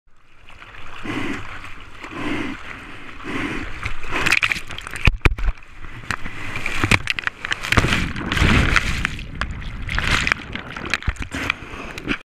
Surfing in Ventura County, California Quick Tube - New Gopro Hero
Quick Tube in Ventura County, California Surfing. Filmed with New Gopro Hero with Mouth Piece.